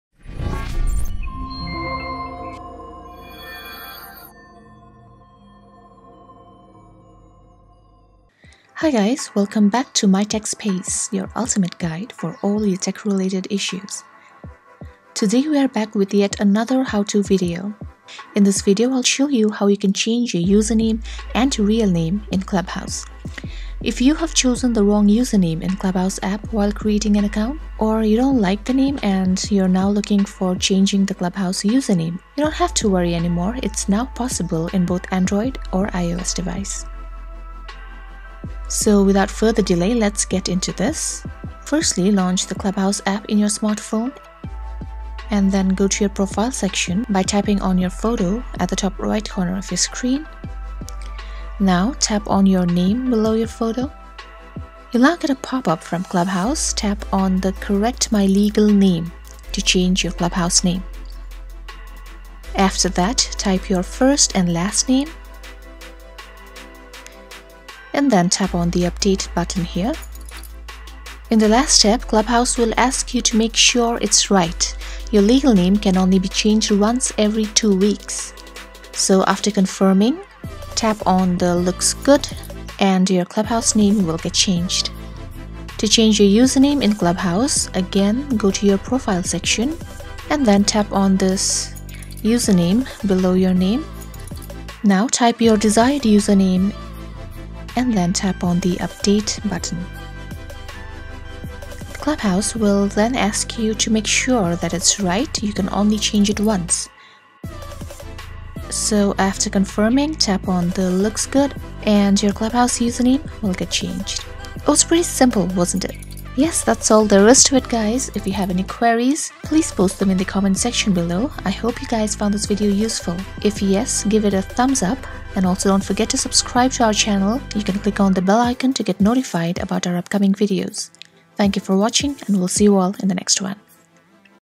How to Change Profile Name and Username on Clubhouse for Android | Can't Change Name Problem Fixed
Problem changing username and display name on Clubhouse for Android and iOS in 2021, Don't worry we got you all covered. In this video, we'll be discussing the step wise process of how to change username and profile name on Clubhouse. Hope this video will be useful to you.